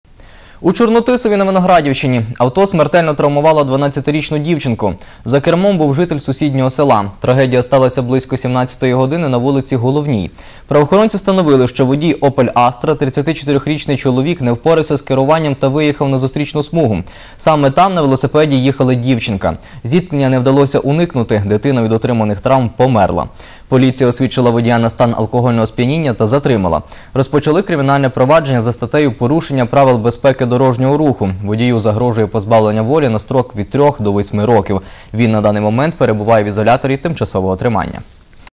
У Чорнотисові авто смертельно травмувало 12-річну дівчинку
У Чорнотисові на Виноградівщині авто смертельно травмувало 12-річну дівчинку. За кермом був житель сусіднього села. Трагедія сталася близько 17-ї години на вулиці Головній. Правоохоронці встановили, що водій «Opel Astra», 34-річний чоловік  не впорався з керуванням та виїхав на зустрічну смугу. Саме там на велосипеді їхала дівчинка. Зіткнення не вдалося уникнути, дитина від отриманих травм померла. Поліція освідчила водія на стан алкогольного сп’яніння та затримала. Розпочали кримінальне провадження за статтею порушення правил безпеки дорожнього руху. Водію загрожує позбавлення волі на строк від трьох до восьми років. Він на даний момент перебуває в ізоляторі тимчасового тримання.